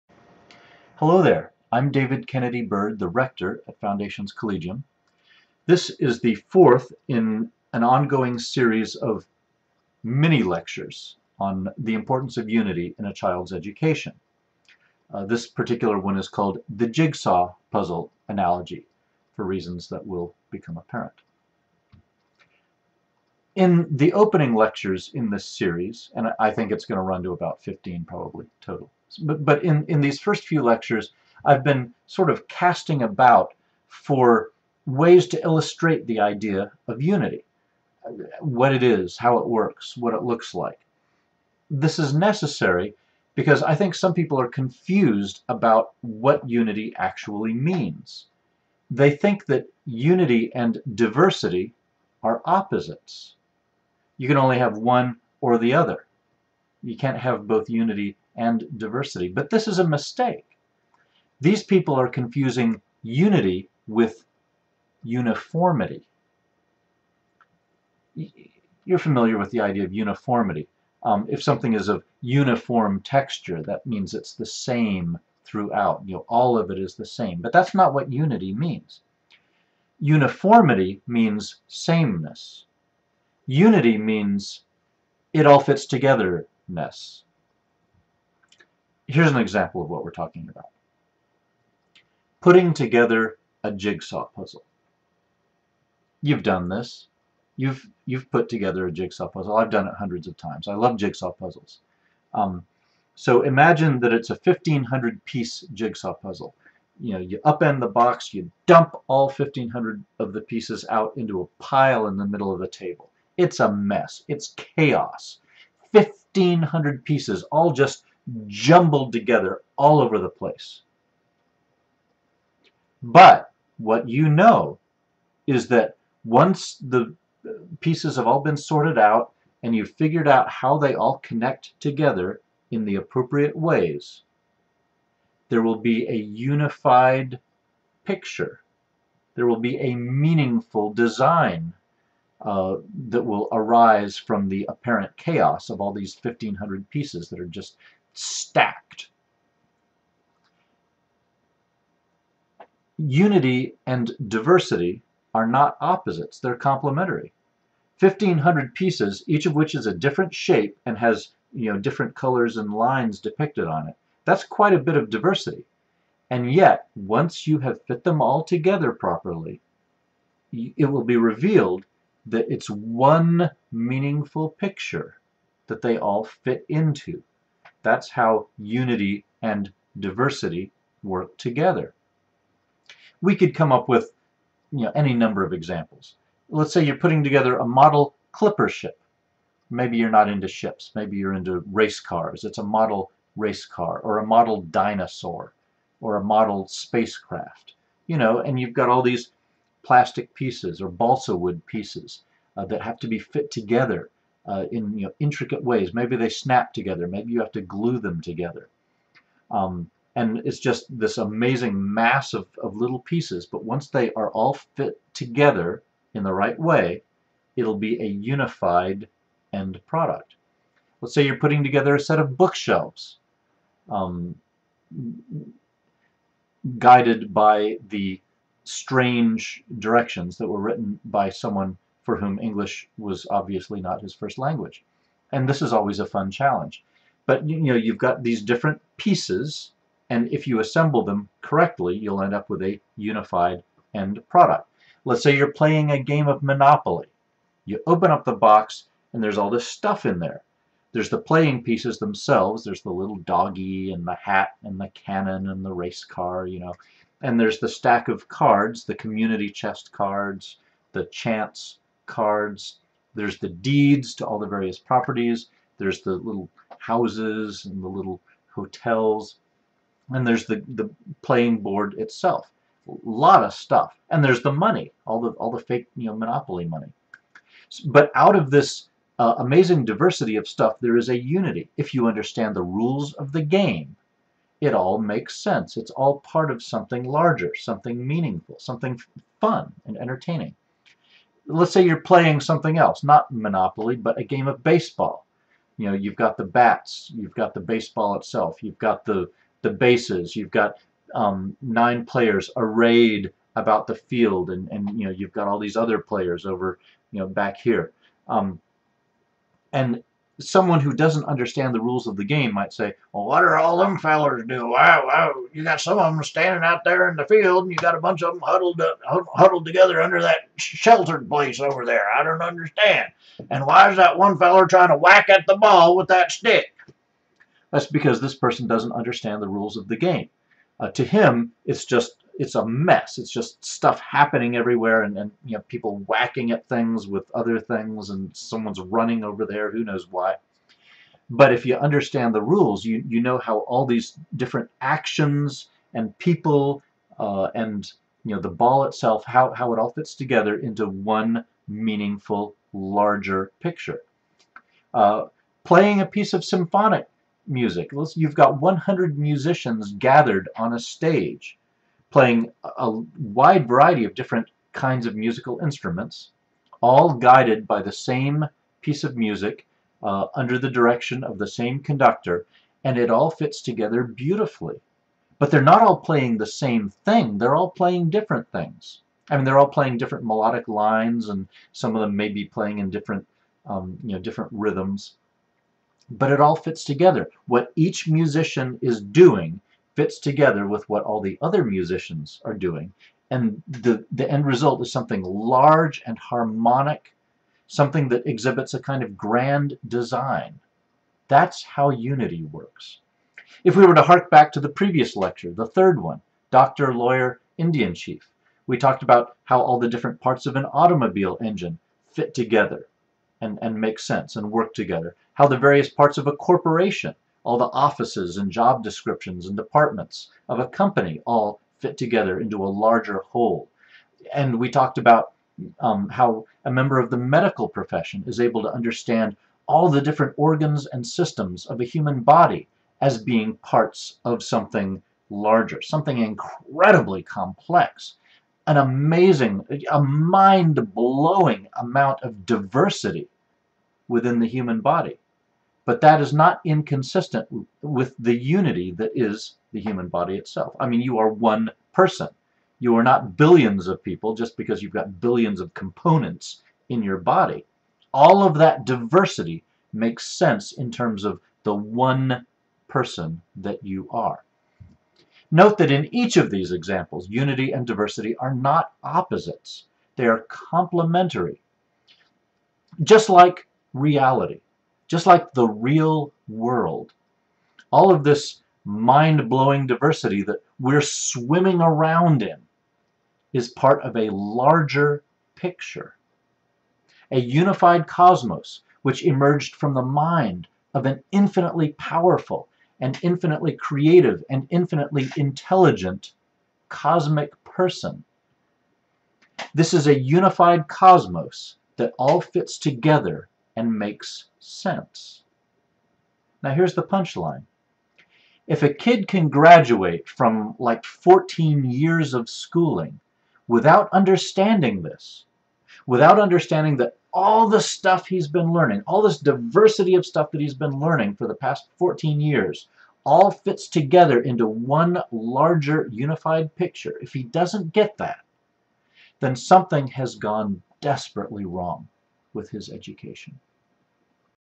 The Importance of Unity in Education (Part Four): The Jigsaw Puzzle Analogy
Looked at in a certain way, it's just a jumbled pile of meaningless pieces in the middle of a table.  However, if you understand the concept of a jigsaw puzzle, you'll realize that all of those pieces fit together into a meaningful Larger Picture.  And that [...sigh...] is what education's supposed to be like.  But how many students have graduated high school without ever having the experience of seeing the pieces fit together into a cosmic big picture that makes sense?